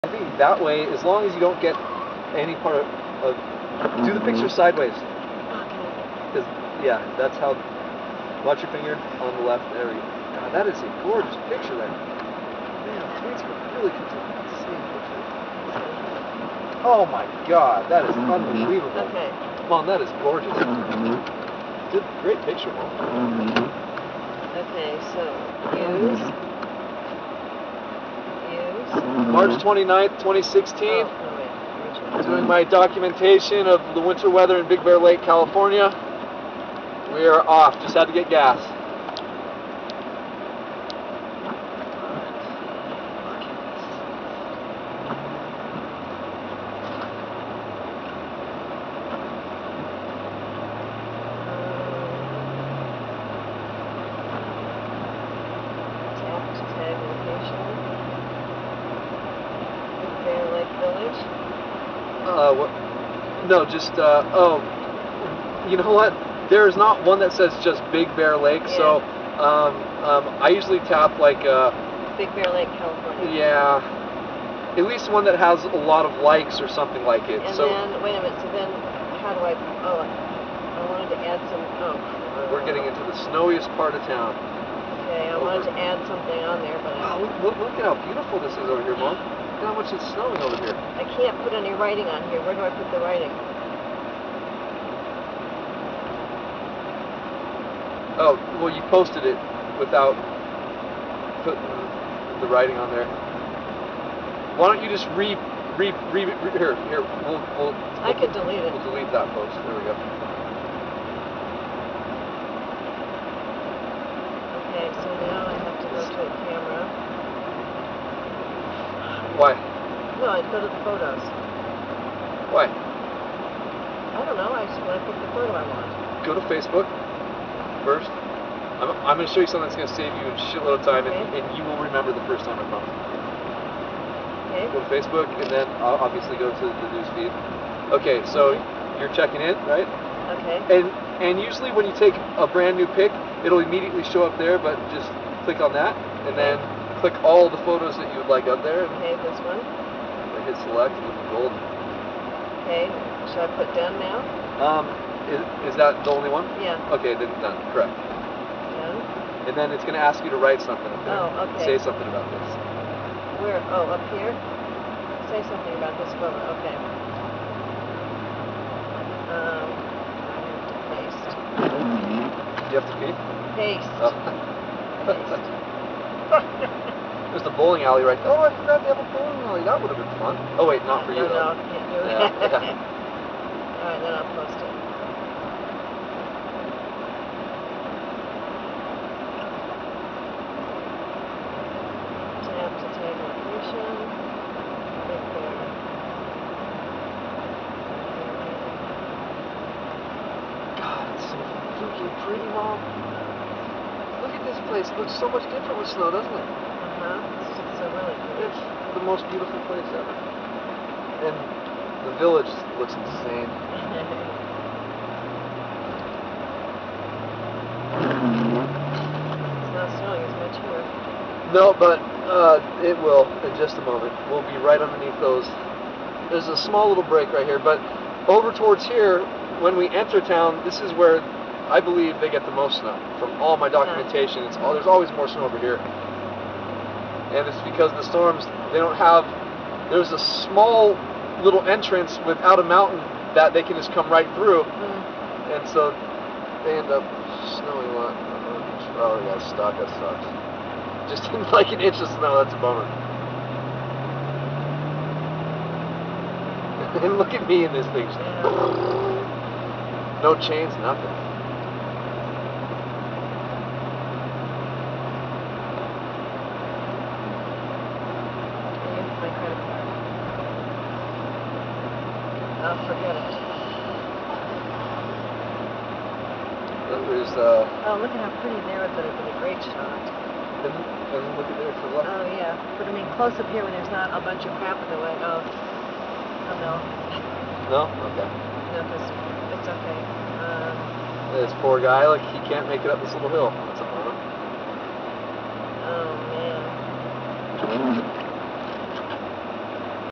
Big Bear Lake, CA driving around with new snow. March 29, 2016 Part 3
Driving from Big Bear Lake, CA Down the bottom on Highway 18 and Highway. 330. Super foggy and raining and snowing.
Its March 28, 2016 Part